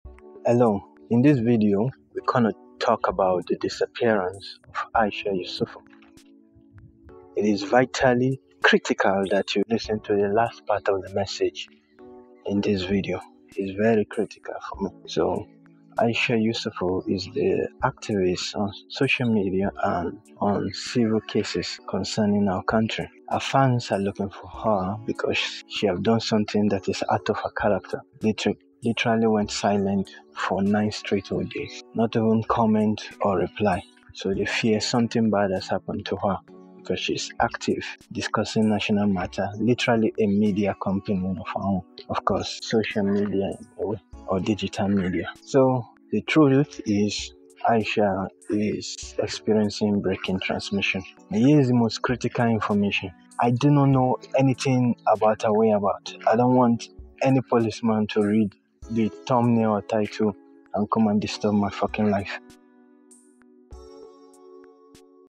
Aisha Yesufu Disappears From Social Media ~ Here's What We know ~ As Fans Demand Answers
Why Aisha Yesufu hasn't posted in DAYS! Here is what we know as friends demand answer.